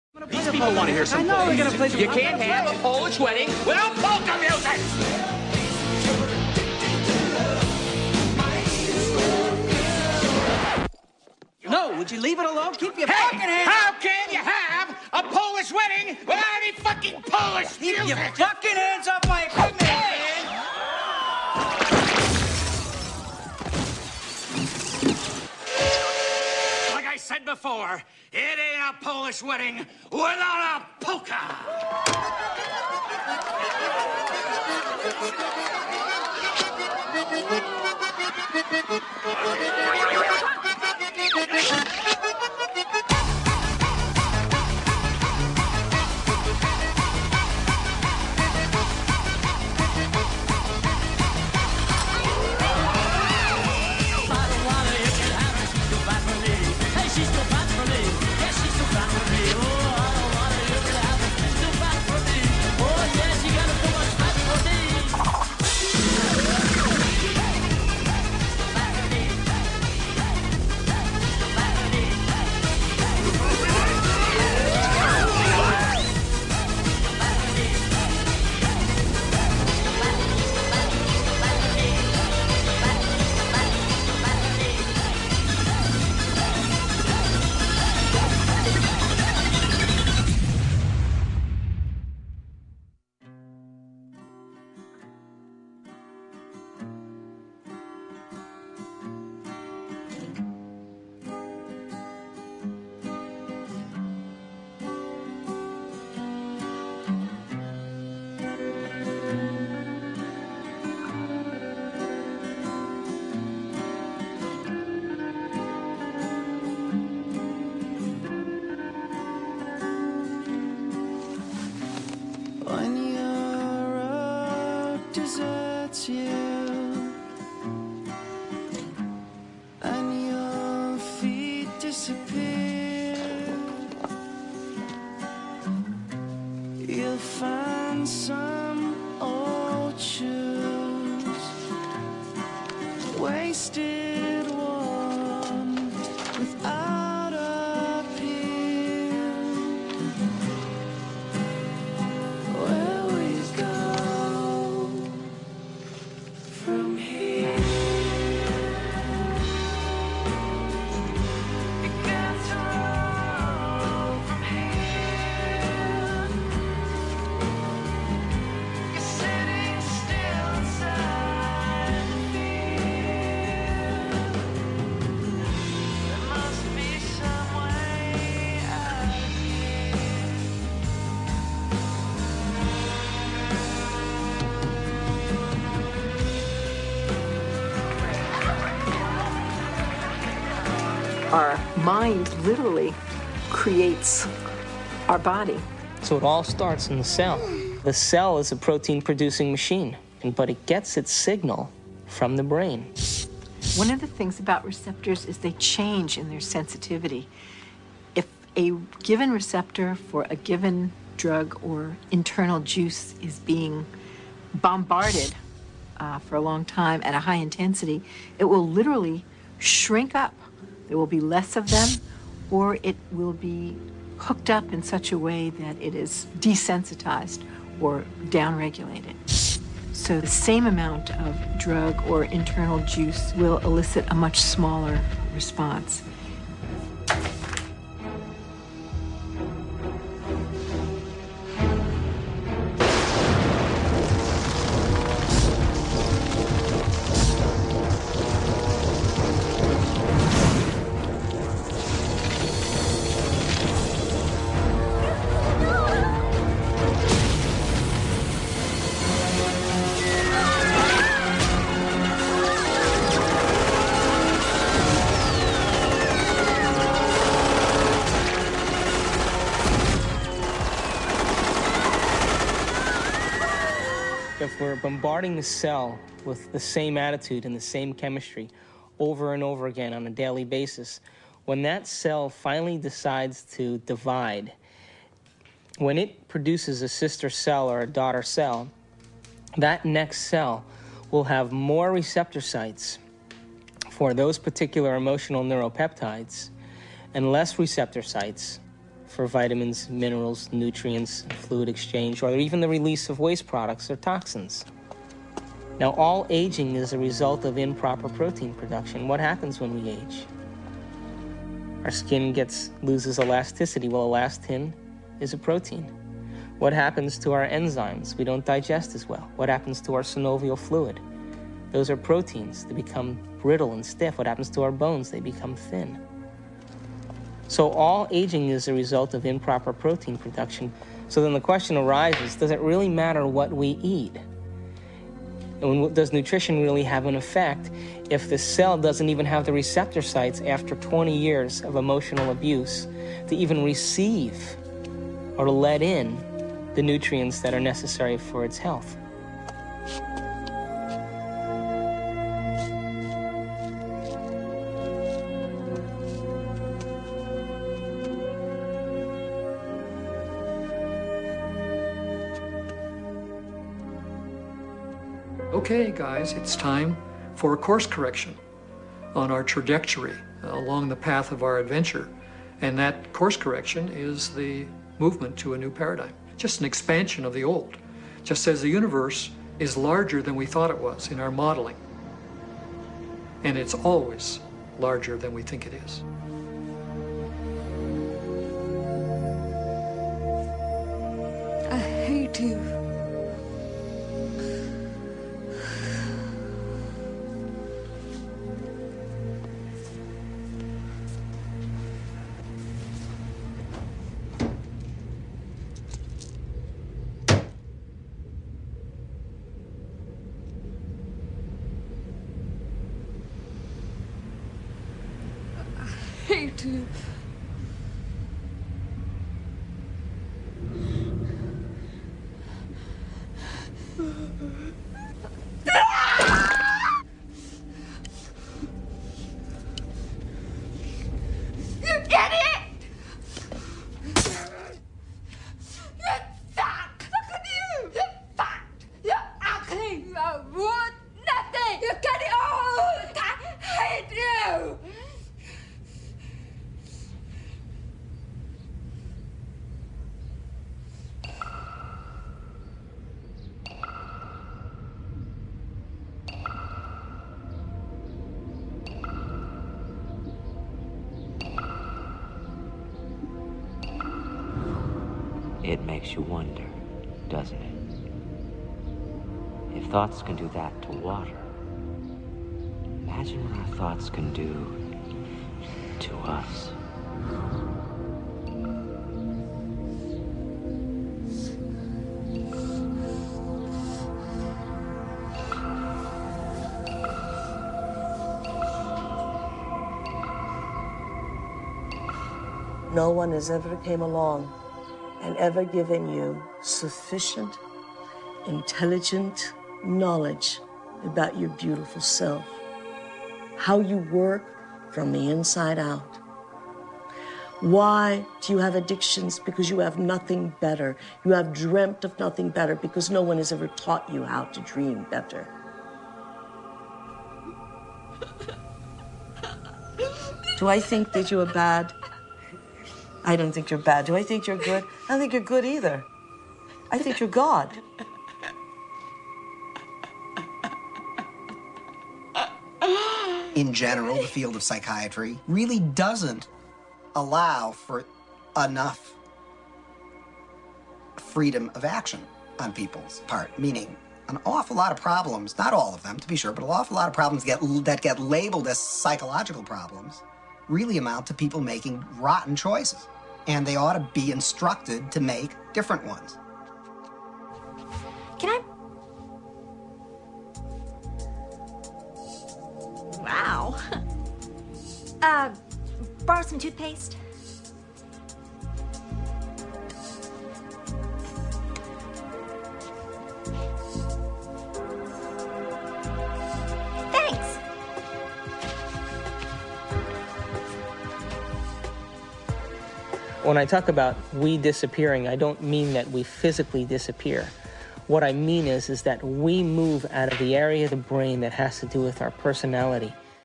Quantum Physic applied to Mind Power - Part 8 of 10
Philosophers, gurus, yogis, thinkers, masters, etc..., thoughts their empirical thinkings were thruths. They are today validated by quantum physic. We are all mighty : proof in the videos...enjoy.